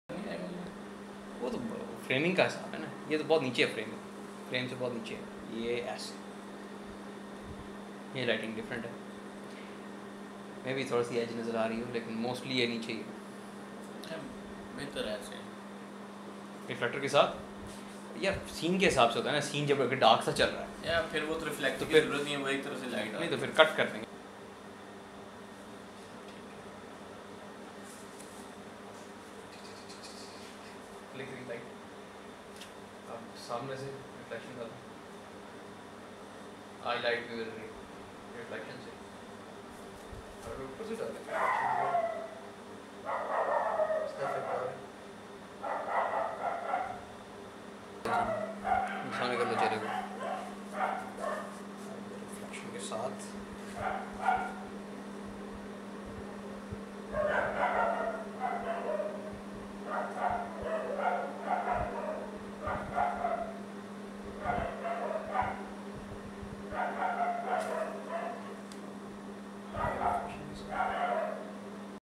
PRE-PRODUCTION - Performing lighting and look tests for an upcoming project
Just showing off a piece of my pre-production process, testing the lighting, lenses and the grading for an up coming project...

Hassaan Khalid having fun painting with lights.